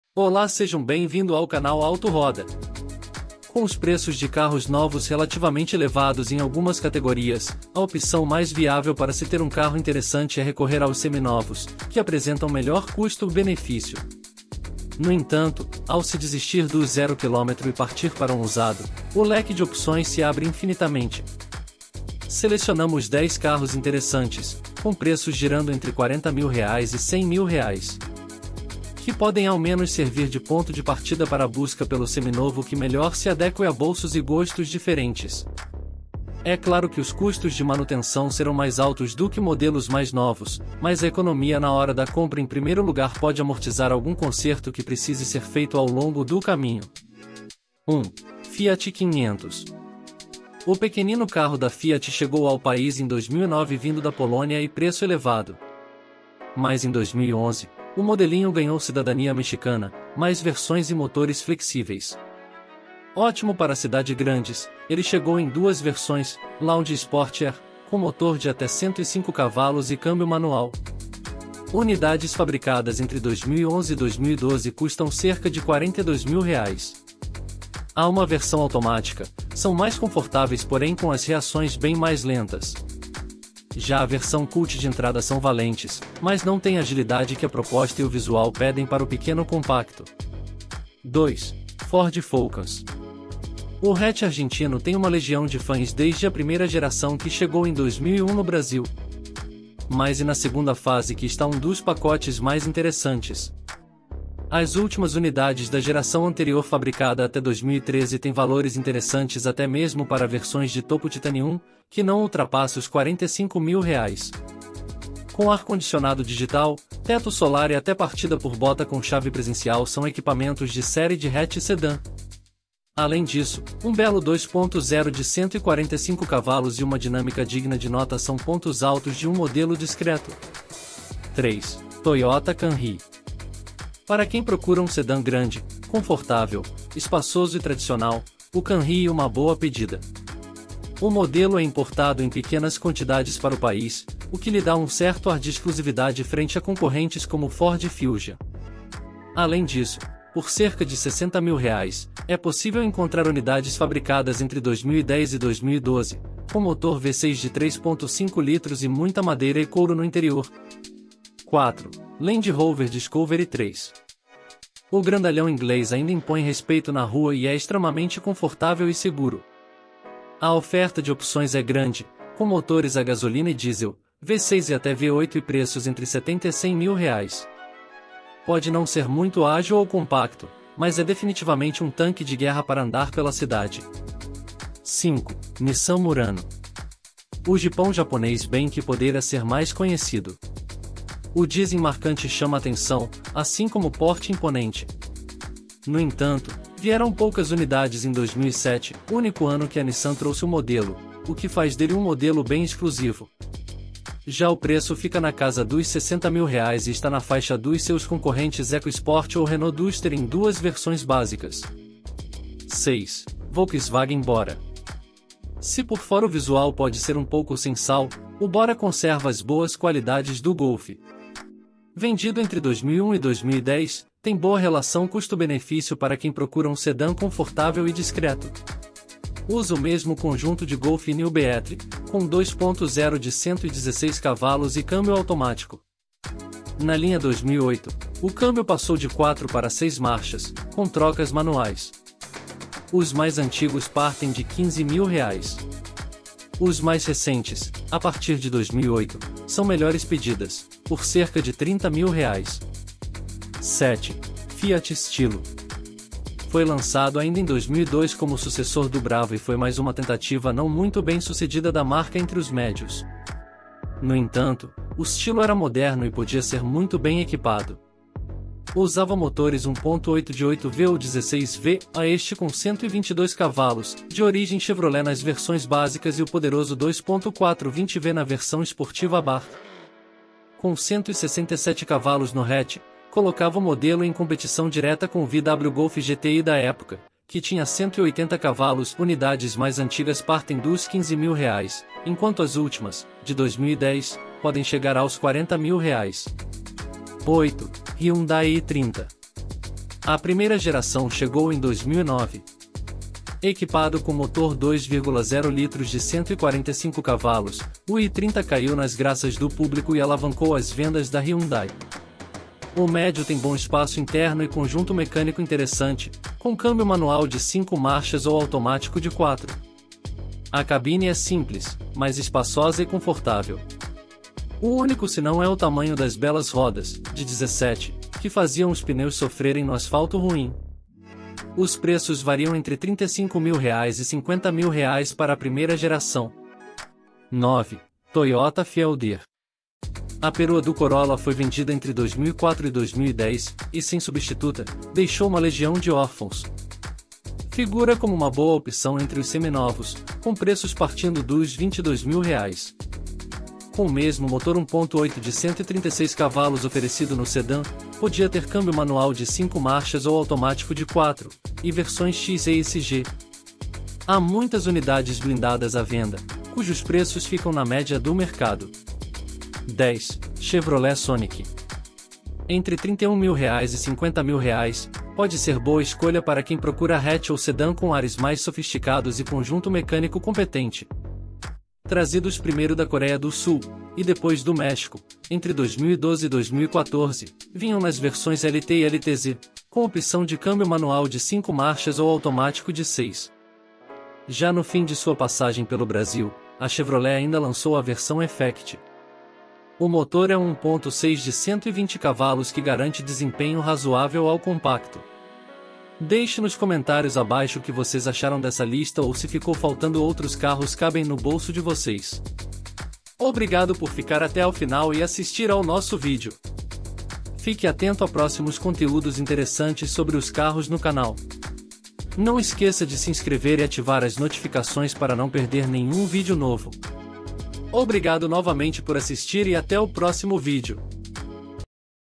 DESCUBRA quais são os 10 CARROS que CABEM no seu BOLSO!!
Se você está pensando em trocar de carro, está com a grana curta mas quer adquirir um automóvel bom, bonito, econômico e que não incomode então você veio ao lugar certo! Essa lista é baseada em carros que estão a venda no mercado de usados em 2022 e inicio de 2023, ou seja, os preços atuais que você vai encontrar.
Nesse vídeo colocamos diversos carros e de categorias diferentes. Mostrando os seus pontos positivos e negativos.